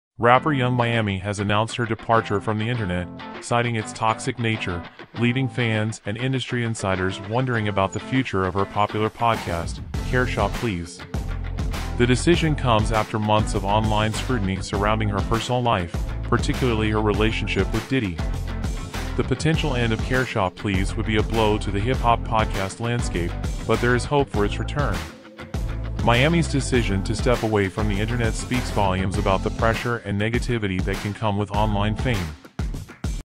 Yung Miami Steps Back from Internet, Fueling Speculation about "Caresha Please" Podcast
Yung Miami has announced her departure from the internet, citing its "toxic" nature, leaving fans and industry insiders wondering about the future of her popular podcast, "Caresha Please." The show, known for its candid conversations, has gained a loyal following and critical acclaim.